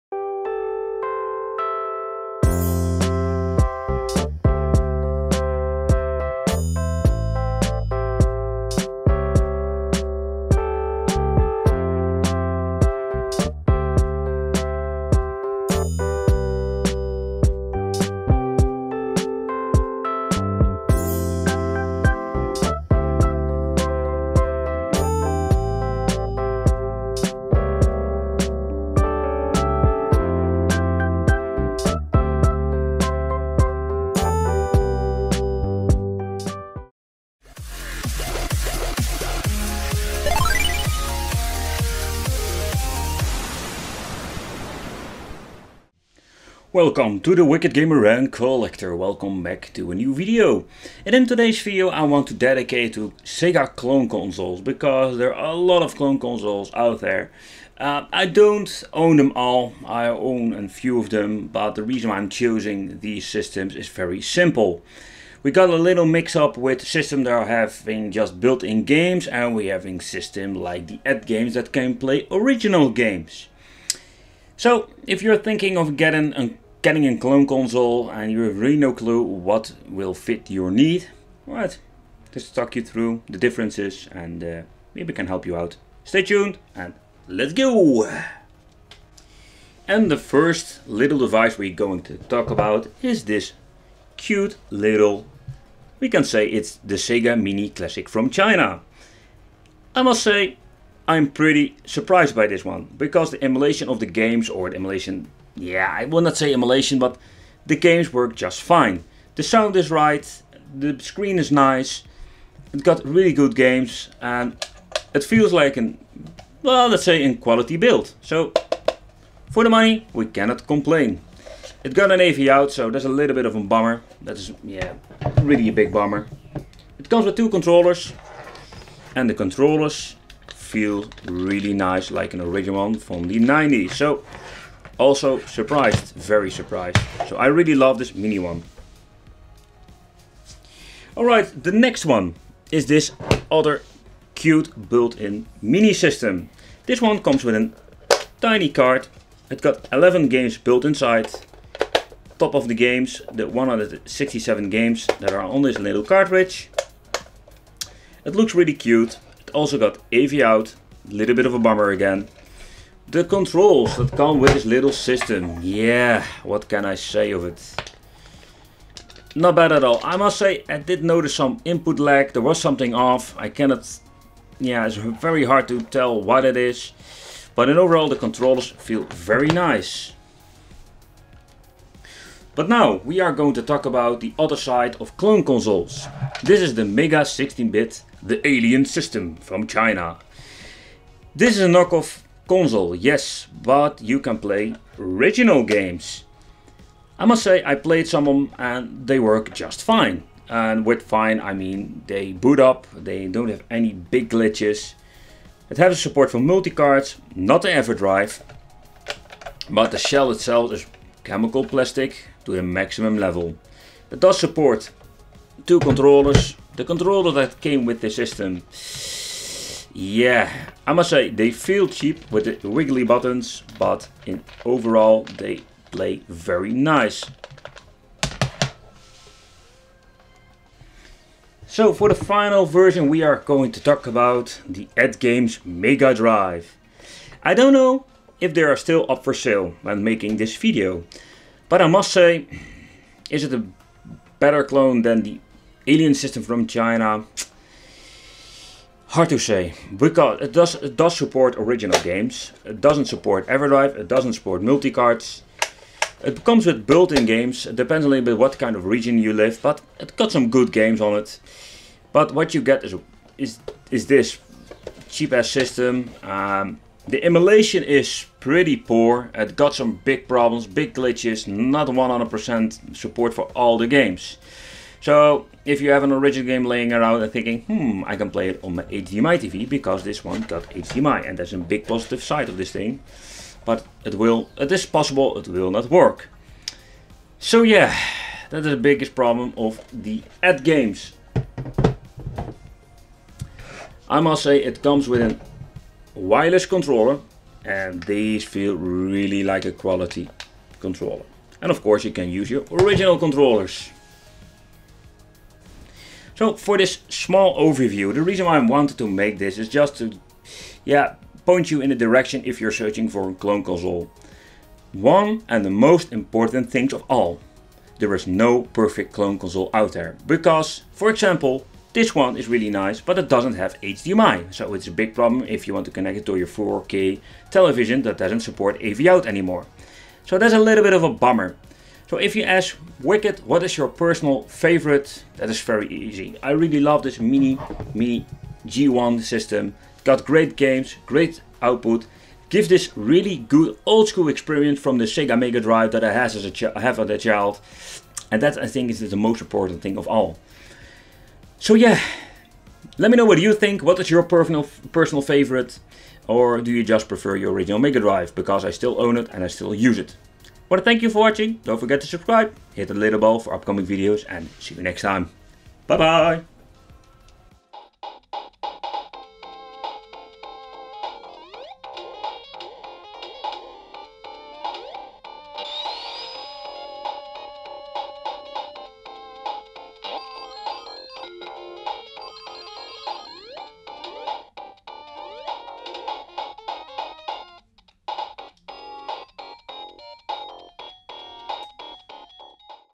Sega Mini Classic China Console Comparison Video Review | At Games
Sega Mini Classic / China Multi Game Console / Retro Gaming / VideoGame  / Games / NES / SNES / Collection / Collections /

Where to find products like this ?
Sega Alien Tech
Sega G1 Clone
Sega 167 in 1

Support me the Wicked Gamer & Collector on Youtube !